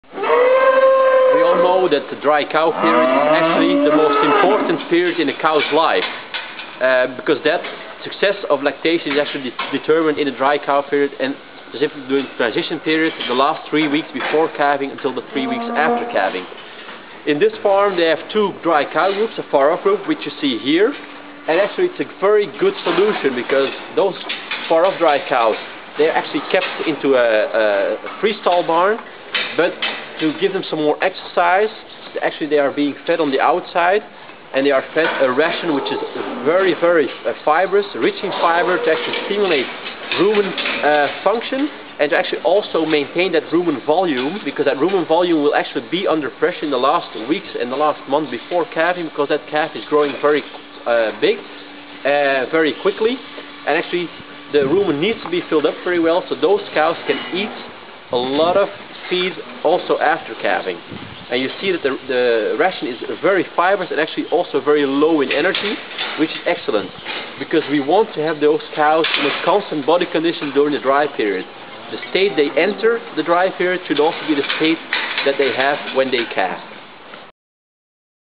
Dry cow feeding and housing in a nutshell.avi
Tom Vanholder explains housing of dry cows and the importance of a fibrous ration for this far off group